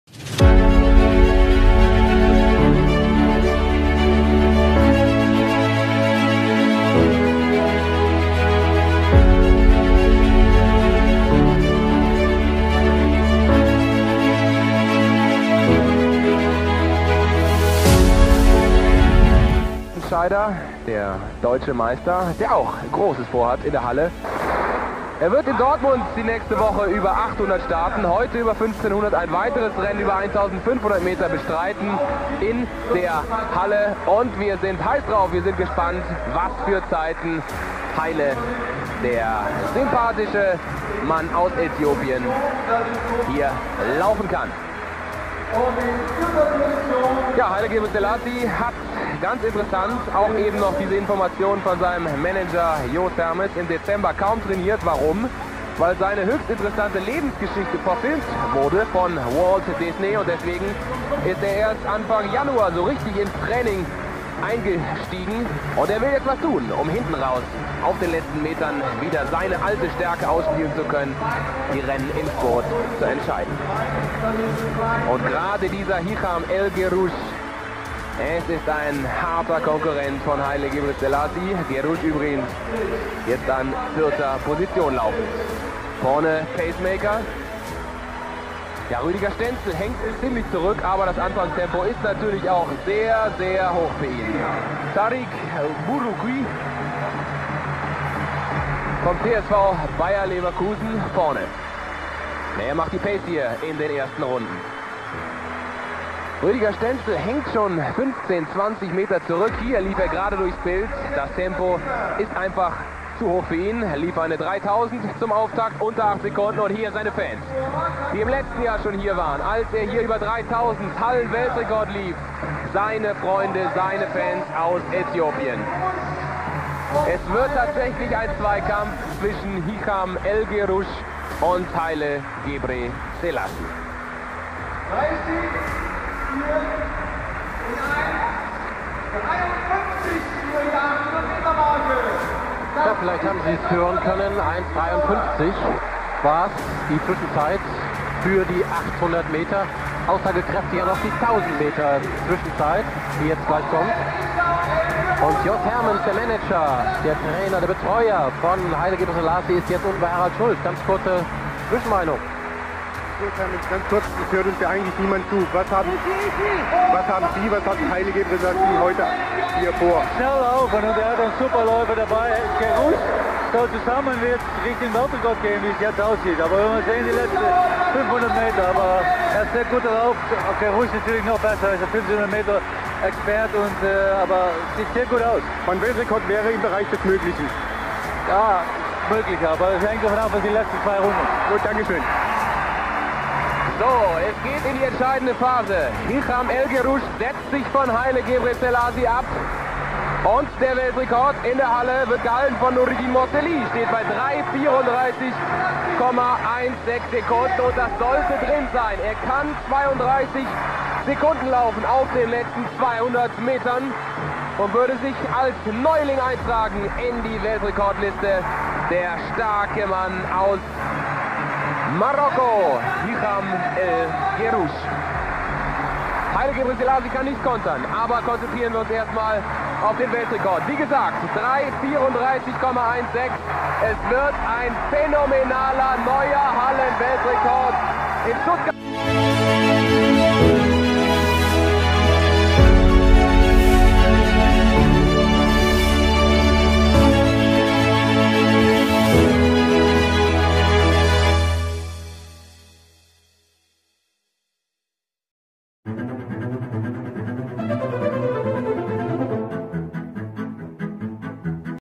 يوم تنافس الجبارين هشام الكروج و جبريل سيلاسي على تحطيم الرقم القياسي لمسافة 1500 متر داخل القاعة .
ـــــــــــــــــــــــــــــــــــــــــــــــــــــــــــــــــــــــــــــــــــــــــــــــــــــــــــــــــــــــــــــــــــــــــــــــــــــــــــــ

في هذا الفيديو المذهل لسباق 1500 متر ضمن بطولة كأس سباركسن شتوتغارت في 2 فبراير 1997، تنافس العداء الأسطورة هشام الكروج والعداء الإثيوبي الأسطورة هايلي جبريل سيلاسي في منافسة مثيرة.

بدأ السباق بحذر من العدائين، حيث سيطرت طفيفًا على المقدمة هشام الكروج. قام جبريل سيلاسي بمطاردة شرسة ومحاولة التفوق على منافسه. لكن في منتصف السباق، بدأ هشام الكروج يزيد من وتيرته بشكل ملحوظ، متجاوزًا إيقاعه السابق.

على الرغم من جهود العداء الإثيوبي القوية، إلا أن هشام الكروج استمر في توسيع الفارق بينه وبين منافسه. أظهر قوته وتفوقه اللافت في النصف الثاني من السباق. وفي نهاية المطاف، تمكن هشام الكروج من الفوز بفارق شاسع على هايلي جبريل سيلاسي.

بفضل أدائه المذهل، نجح هشام الكروج في تحطيم الرقم القياسي العالمي لمسافة 1500 متر داخل القاعة. كانت هذه اللحظة تاريخية ولا تُنسى في مسيرة هشام الكروج كأحد أعظم العدائين في التاريخ.

إن الفيديو يسلط الضوء على التنافس الشرس والأداء الاستثنائي لهشام الكروج وهايلي جبريل سيلاسي، مما يجعله مشهدًا لا ينسى في عالم ألعاب القوى